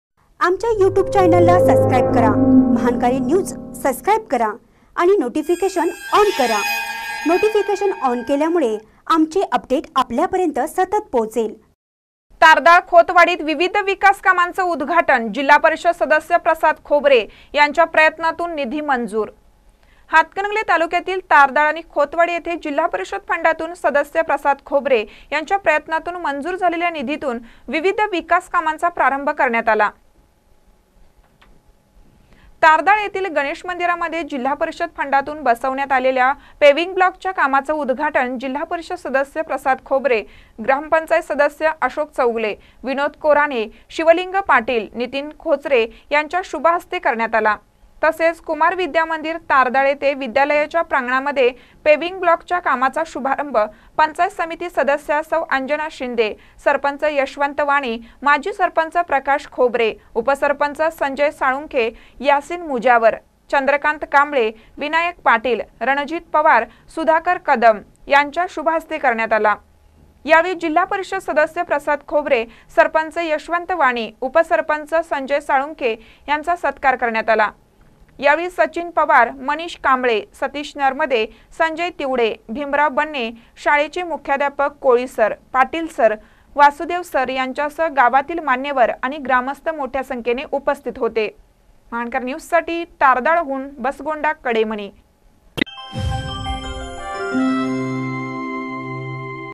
तारदाळ, खोतवाडीत विविध विकास कामांचे उद्घाटन
तारदाळ, खोतवाडीत विविध विकास कामांचे उद्घाटन : जिल्हा परिषद सदस्य प्रसाद खोबरे यांच्या प्रयत्नातून निधी मंजूर